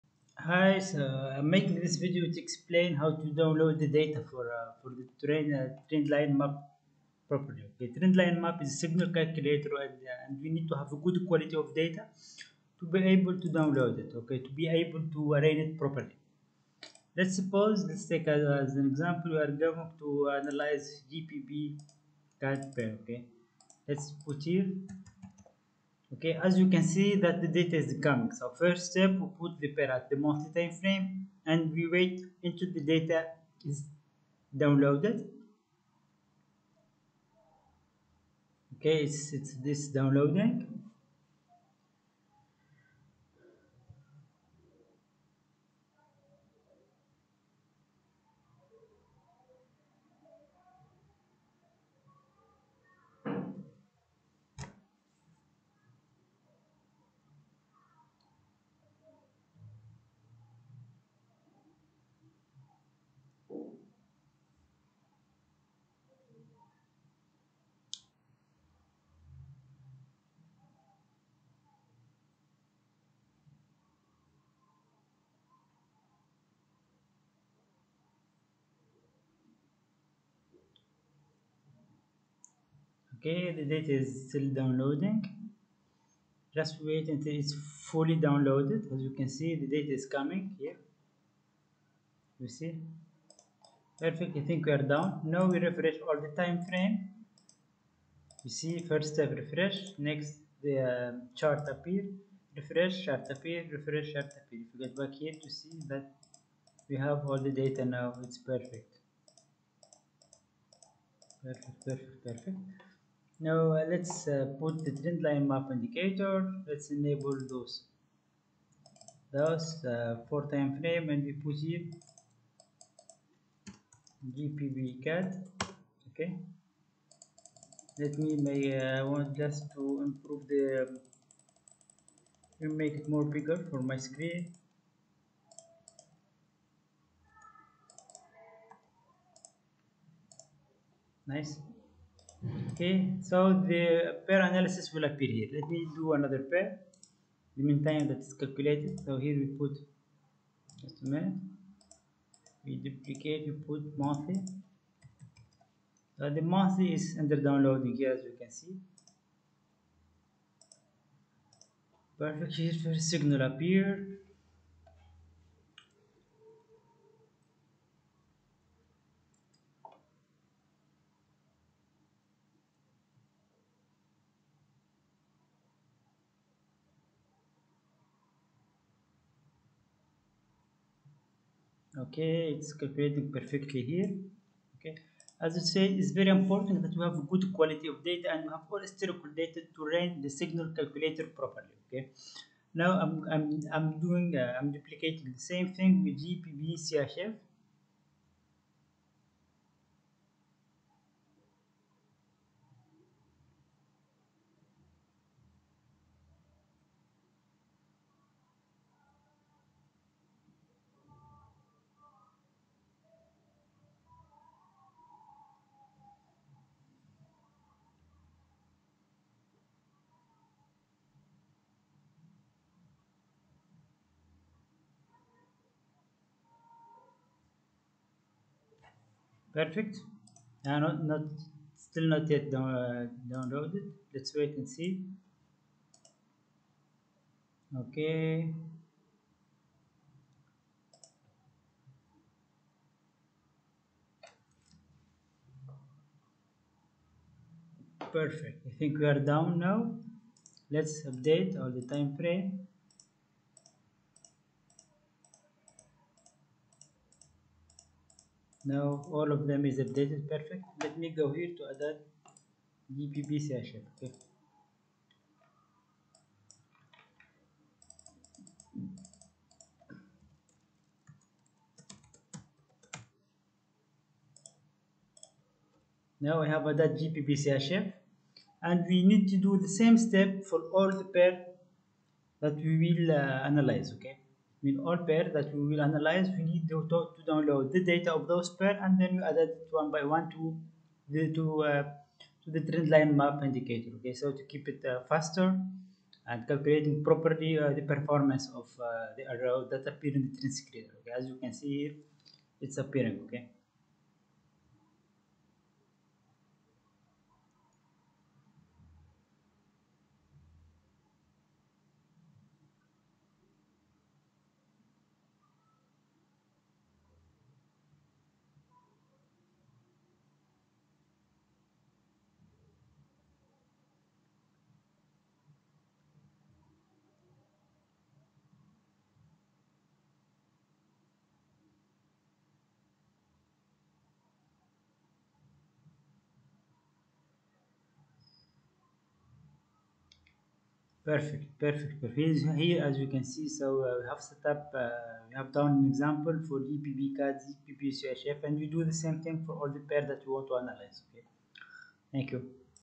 Download Historical data for MT5
Download Historical for trend screener and Trend line map indicator.